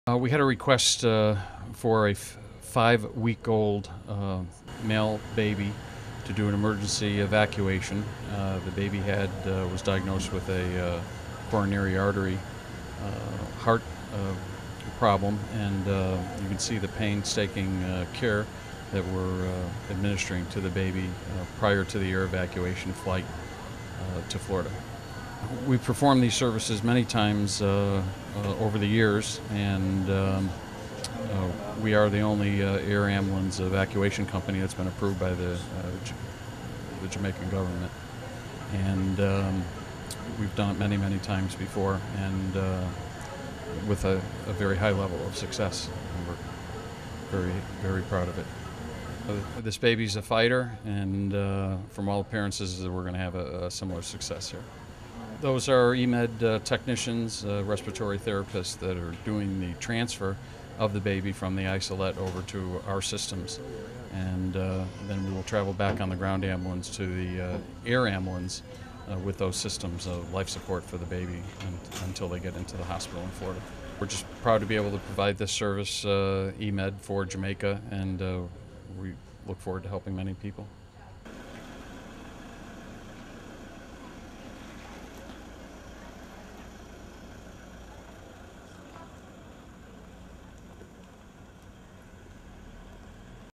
EMED AIR AMBULANCE CAYMANS
Duane Boise, president of EMED air ambulance narrates the specialized procedures used in EMED's pediatric medical evacuation services provided through out the Caribbean.